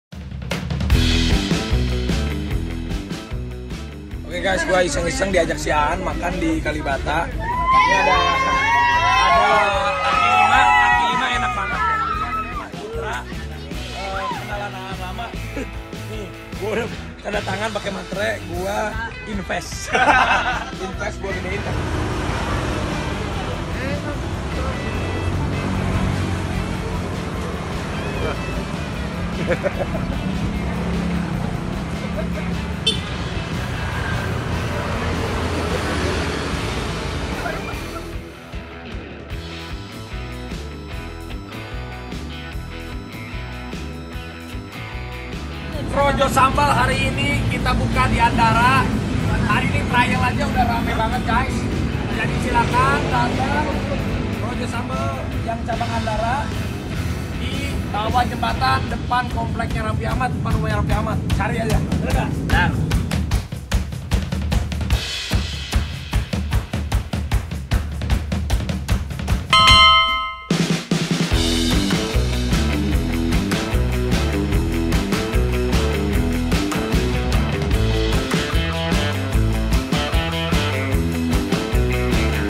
Keenakan Makan Bebek Kaki Lima, Raffi Ahmad Langsung Tanam Modal di Tempat
Gak pake banyak mikir, Raffi Ahmad langsung ingin investasi di tempat setelah jatuh cinta dengan rasa bebek asap di sebuah rumah makan kaki lima.

Sultan Andara emang beda ya😂

Baca selengkapnya di hypo.id ✨